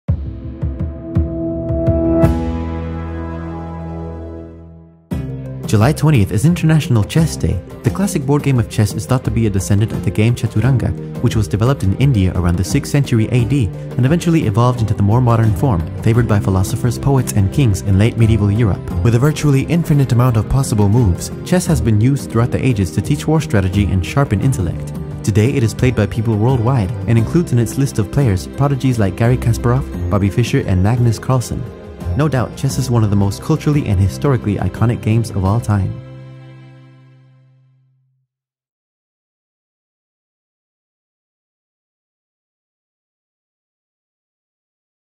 July 20th-International Chess Day
Ep. 4 of Syne Shorts, "Calendar": International Chess Day.

"July 20th is International Chess Day
The classic board game of chess is thought to be a descendant of the game Chaturanga, which was developed in India around the 6th century AD and eventually evolved into the more modern form favored by philosophers, poets, and kings in late-medieval Europe.
With a virtually infinite amount of possible moves, chess has been used throughout the ages to teach war strategy and sharpen intellect.
Today it is played by people worldwide and includes in its list of players prodigies like Garry Kasparov, Bobby Fischer, and Magnus Carlsen.
 No doubt, chess is one of the most culturally and historically iconic games of all time."

Pendlum's Pick:
From Chaturanga came Shatranj
And from Shatranj came Chess;
Now humans, AIs, and centaurs alike
Play to see who can play best!

Syne Key words for further research: World Chess Federation; Garry Kasparov; Bobby Fischer; Magnus Carlsen; Wilhelm Steinitz; Chaturanga; Shatranj; Deep Blue; Chess960; Shannon number; centaur chess